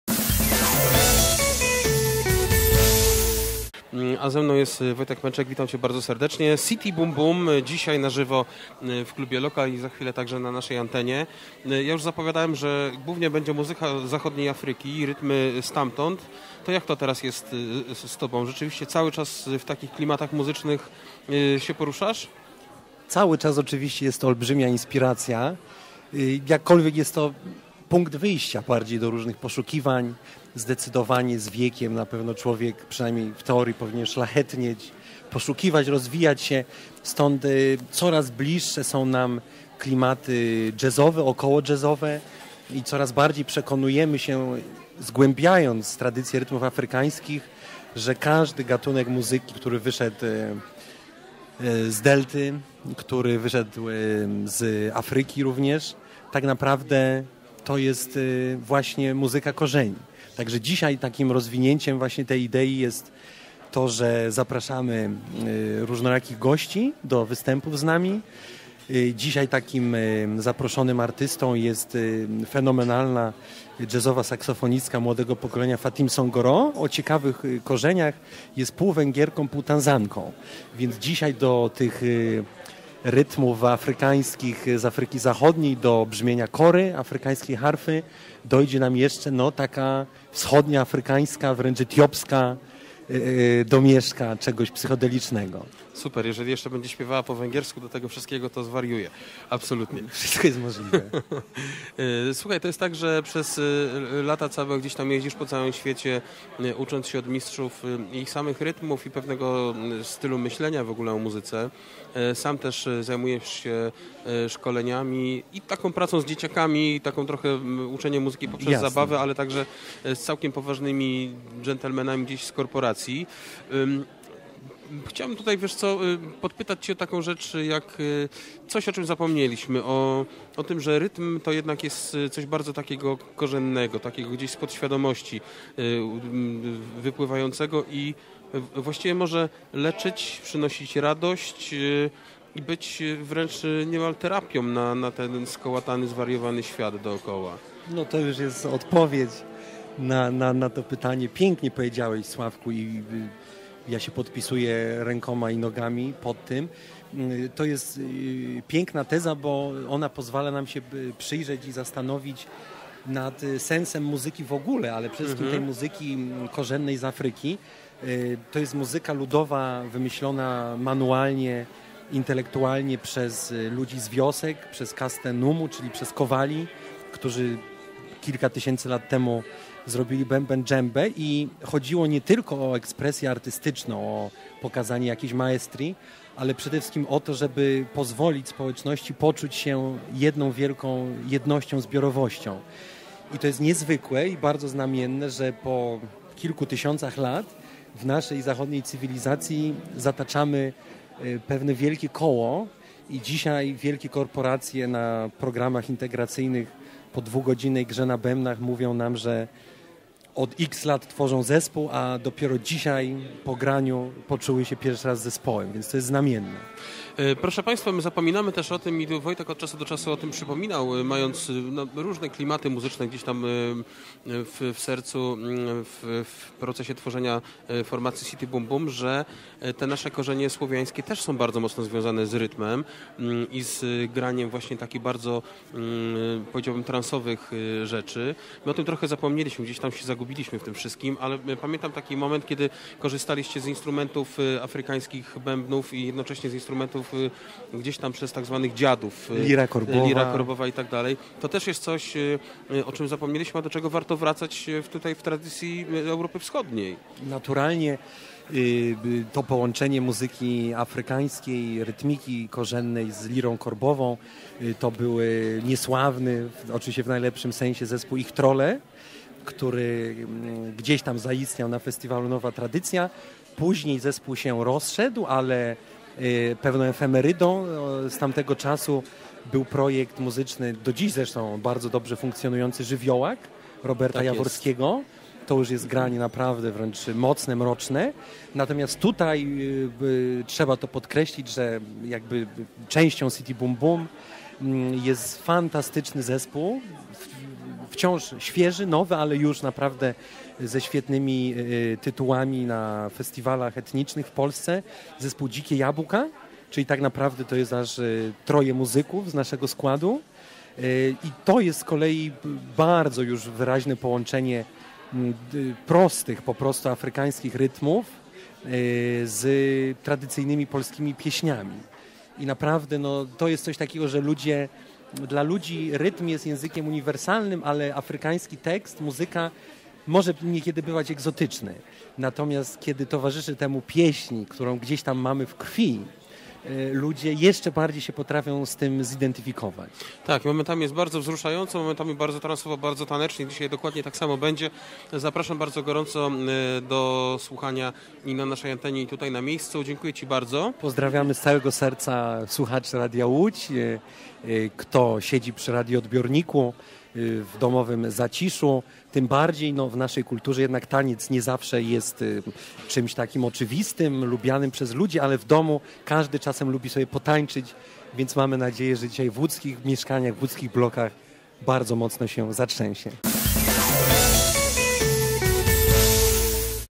Rozmowa z Wojtkiem Pęczakiem z City Bum Bum
W każdy piątek wakacji zapraszamy na LOKALne Letnisko z Radiem Łódź. Co tydzień będą czekać na Was koncerty, sety DJ-skie, konkursy, zabawy i pyszne jedzenie. 8 sierpnia na Letnisku wystąpiła formacja City Bum Bum. Oto rozmowa z jej liderem.